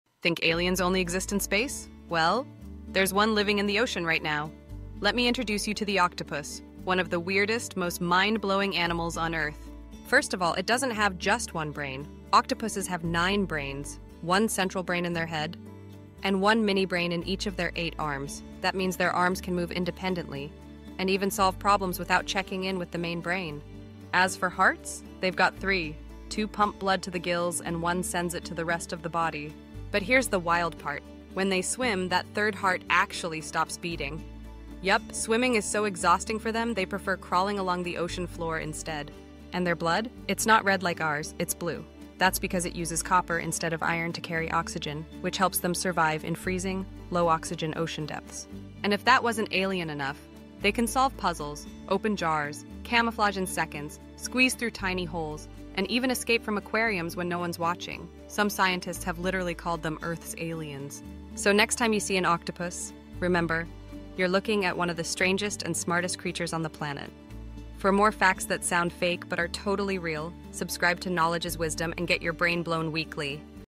Octopuses Are Basically Real-Life Aliens! 🐙👽🌊
🐙💙 Are Octopuses Actually Aliens?! 👽🌊
You might think aliens live in outer space… but what if one of them is already here — swimming deep in our oceans?

Meet the octopus, one of the most bizarre and fascinating creatures on Earth. In this video, we dive into the **weird science** behind why octopuses are often called the aliens of the sea. And trust us — they’ve earned that title! 😱

Here’s what makes them so unbelievable:

🧠 9 Brains!
Octopuses have one main brain and a mini-brain in each of their eight arms. That means they can literally think with their tentacles. Yes — each arm can move, react, and solve problems independently! 🤯

❤️ 3 Hearts!
They have two hearts that pump blood to their gills and one that pumps it to the rest of the body. And when they swim? That third heart actually stops beating — no joke! 😲

🩵 Blue Blood?!
Unlike humans, octopuses have blue blood! That’s because their blood uses copper-based hemocyanin instead of iron, which helps them survive in cold, deep ocean waters. ❄️🌊

Octopuses can camouflage instantly, open jars, escape aquariums, solve puzzles, and even unscrew lids — they’re basically underwater ninjas with a genius IQ! 🧠🐙✨

Some scientists even refer to them as “Earth’s aliens” because they’re so different from every other animal we know.

So the next time you spot an octopus, remember — you’re looking at a creature that’s smarter, stranger, and cooler than almost anything else on this planet.

👇 Drop a comment and tell us:
What would YOU do if you had nine brains?